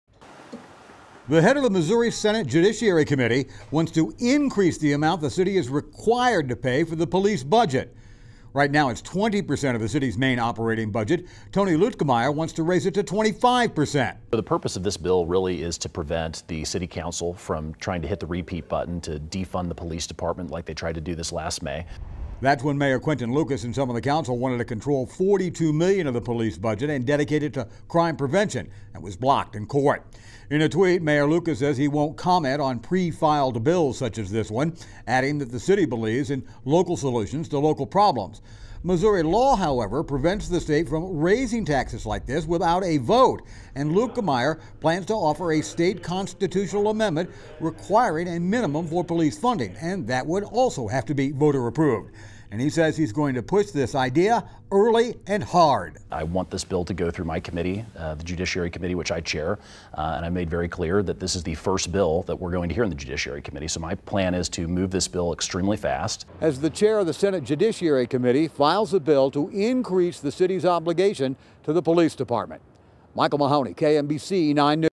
Missouri lawmaker proposes measure requiring KC to increase police funding
The head of the Missouri Senate Judiciary Committee wants to increase the amount Kansas City is required to pay for the police budget.